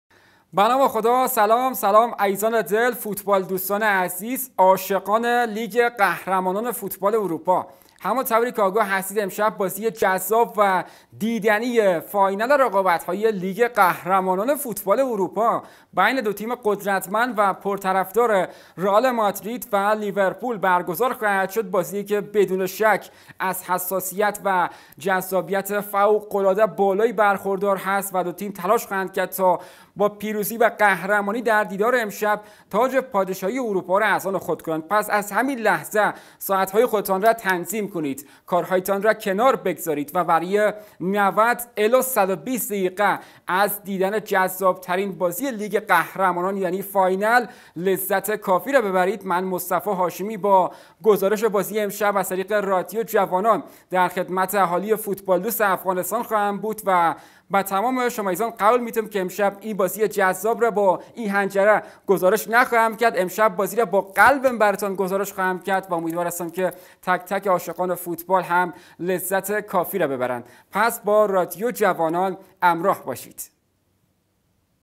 صحبت های مصطفی هاشمی در آستانه فاینل لیگ قهرمانان اروپا
فاینل لیگ قهرمانان اروپا با گزارش مصطفی هاشمی